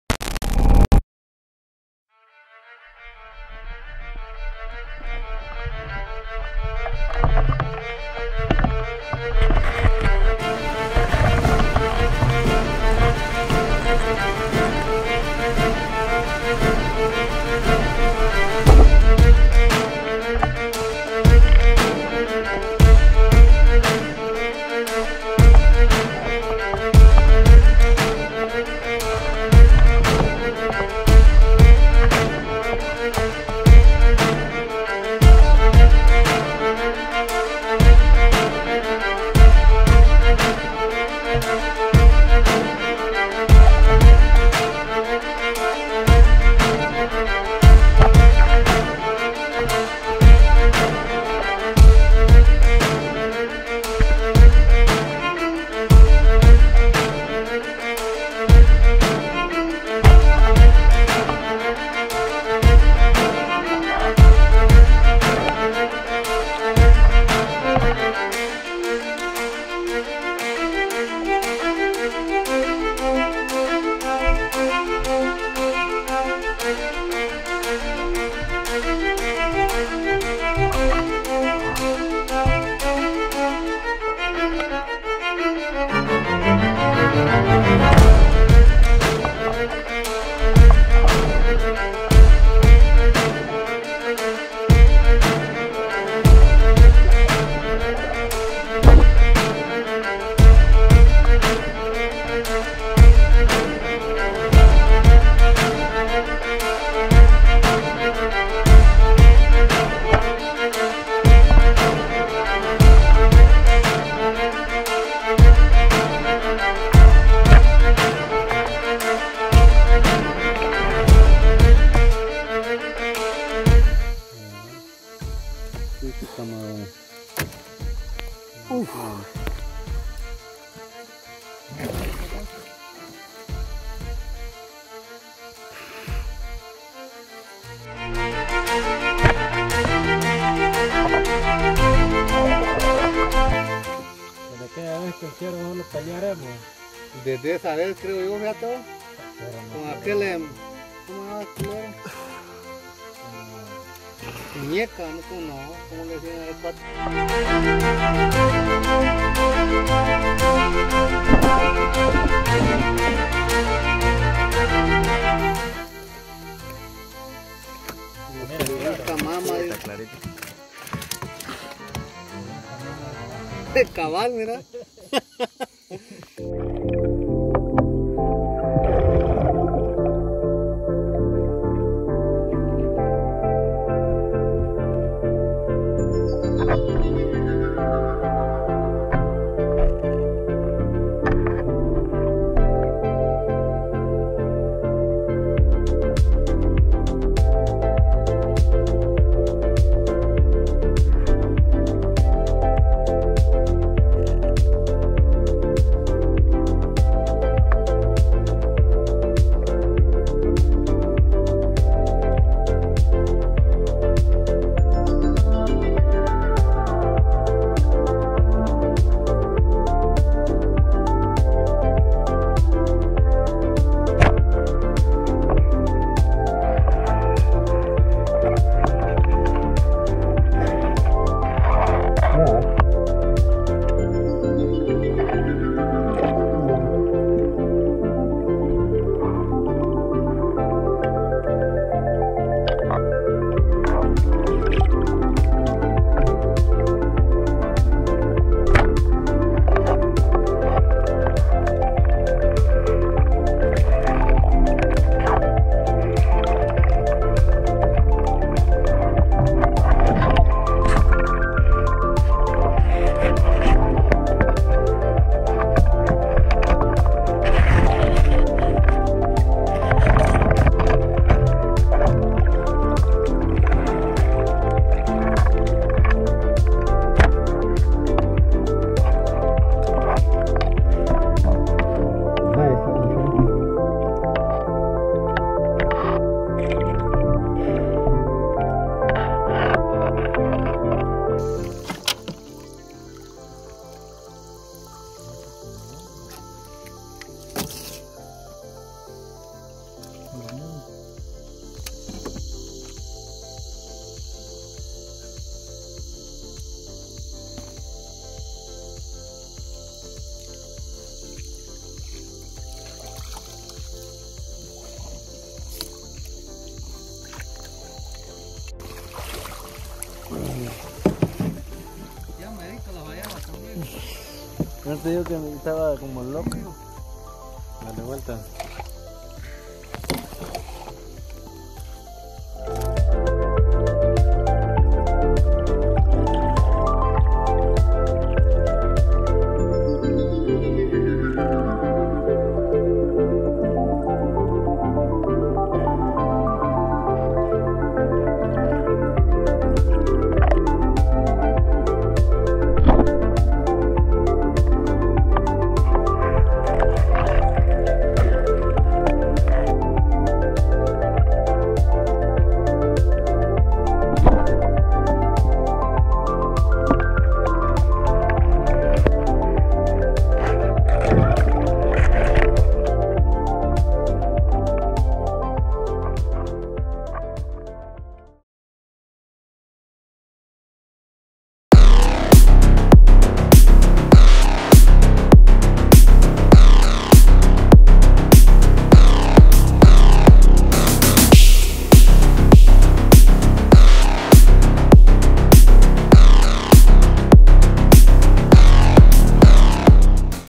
Great Night of Spearfishing In The Belizean Jungle {Big Lobster, Bay Snook, Tilapia, and More}
Today we head out to do some diving in the Belize River. There are fish everywhere and with a little luck we can shoot something BIG.